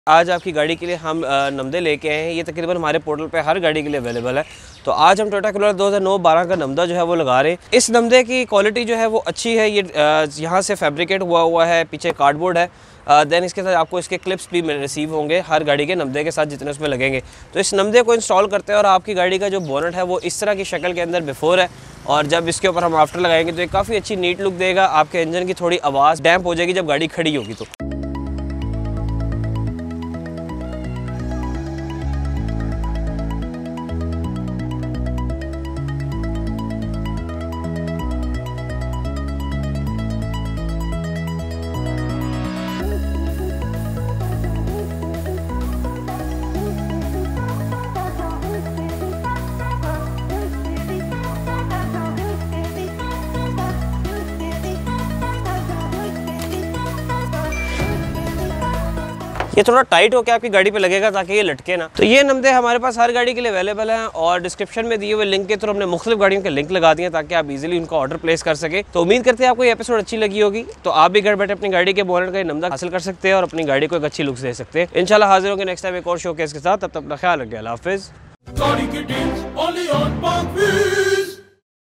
Inner Bonnet Protector Namda | Inner Bonnet Protection Sheet
Inner Bonnet Protector Namda Is Available For Running Models Including Toyota Corolla Old and New, Toyota Vitz, Toyota Aqua, Suzuki Cultus, Honda City, Honda BRV, Wagon R, and Honda Civic.